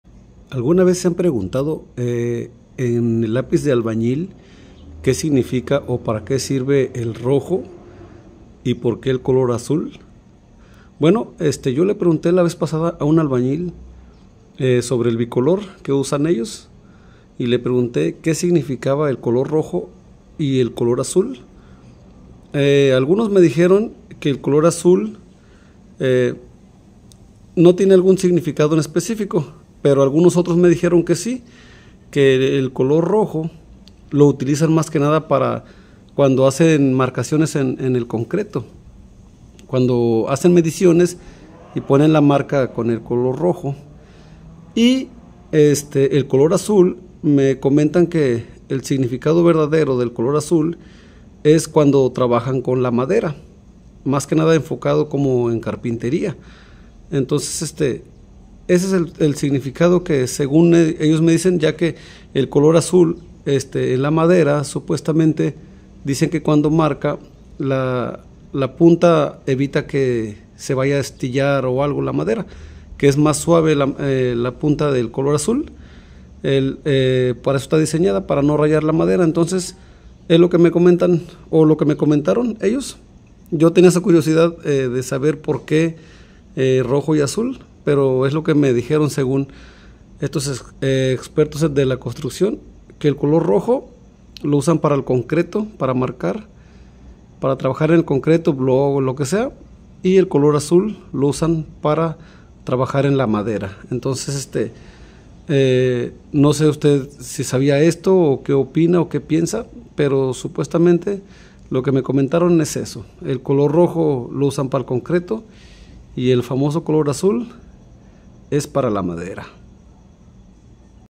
Lápiz de albañil que significa el color rojo y azul
En este vídeo les voy a enseñar una curiosidad sobre el lápiz de albañil,el lápiz de albañil contiene ddos colores rojo y azul y aquí les diré el significado de los colores